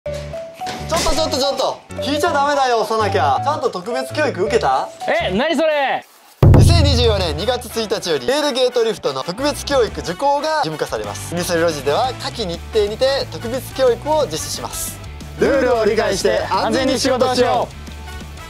テールゲートリフター(パワーゲート)特別教育実施【岡山トレーニングセンター】
テールゲートリフター特別教育受講申し込みページ：
問い合わせ先電話番号：086‐241‐0555(月～土曜日9:00～18:00)
※2023年12月28日木　2024年1月6日、1月8日も追加になりました

岡山トレーニングセンター(運営：ユニセルロジ)では12月23日土曜日にテールゲートリフターの特別教育を行います。
2024年2月1日より、パワーゲートに携わる業務を行う場合特別講習の受講が義務化されました。
まだ受講されていない方はこの機会に受講頂ければと思います。
出張開催・団体教育もご要望の応じて対応いたしますので、お気軽にご相談ください!!

日時：12月23日土曜日 ９:30 ～
場所：講習：岡山南ふれあいセンター　実技：岡山市南区浦安南町243
主催：岡山トレーニングセンター(ユニセルロジ株)
講師:先輩　アシスタント：ショコちゃん